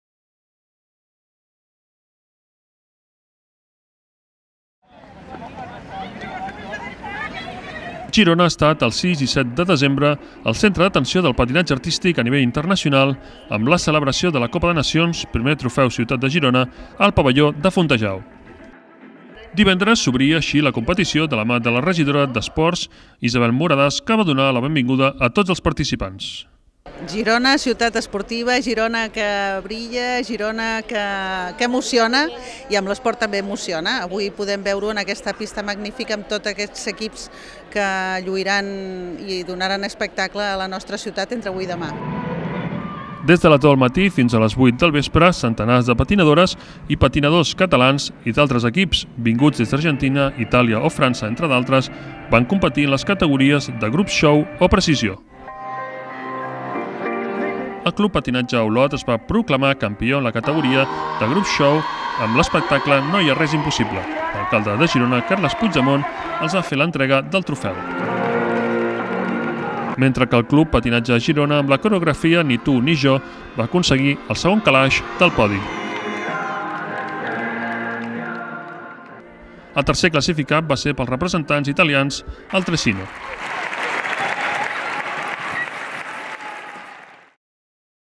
El CPA Olot guanya la Copa de les Nacions de Patinatge Artístic a Girona
El Pavelló Municipal de Girona-Fontajau ha sigut l'escenari aquest cap de setmana de la primera edició de la Copa de les Nacions de Patinatge Artístic en modalitat de grups xous i precisió. A la competició hi han participat 390 esportistes procedents de països com Argentina, França, Itàlia o Espanya. Darrere del grup de Ricard Planiol ha quedat el grup del club organitzador, el CPAGirona, que ha aconseguit el seu millor resultat internacional en el grup de show gran.